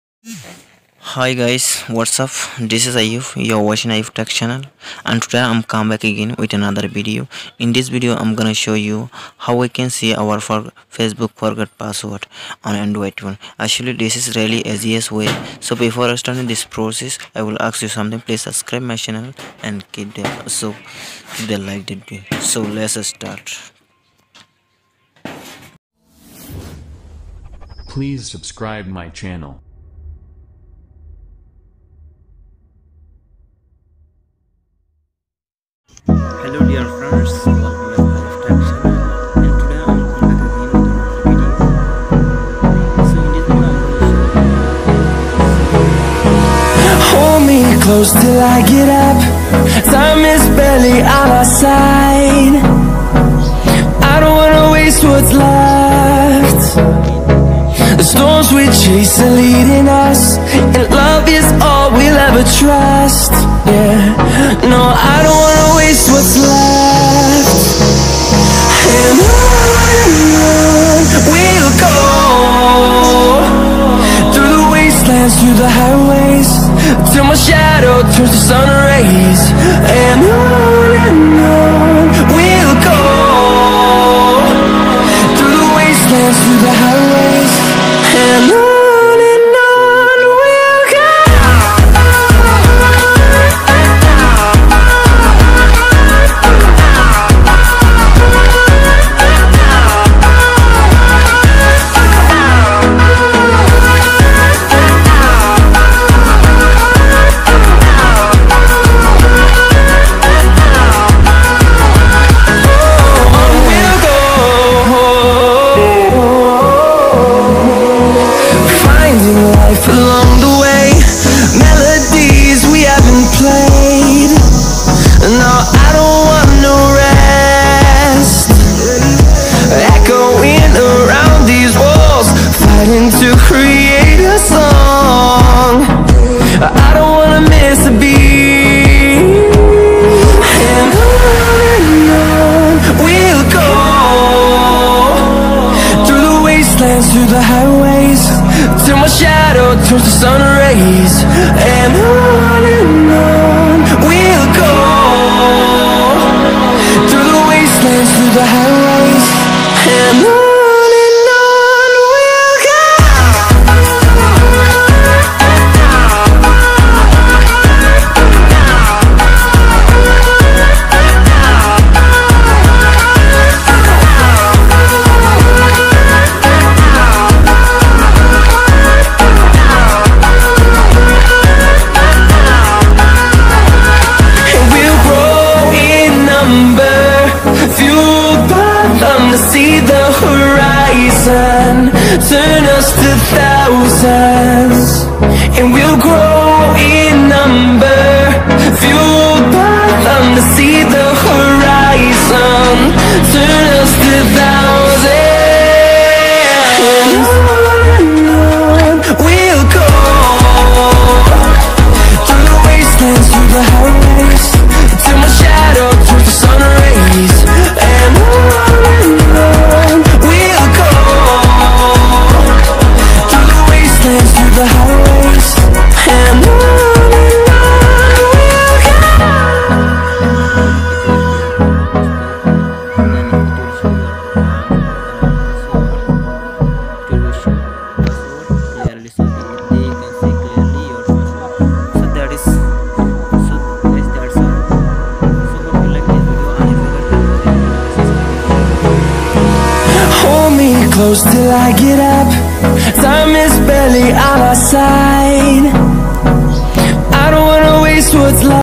How to Recover Forgotten Facebook Password || RECOVER FORGET FB PASSWORD
In this video I am going to show how to recover your forgotten facebook password. If you want to recover your facebook account, keep watching the video till the end .it's very simple and easy process.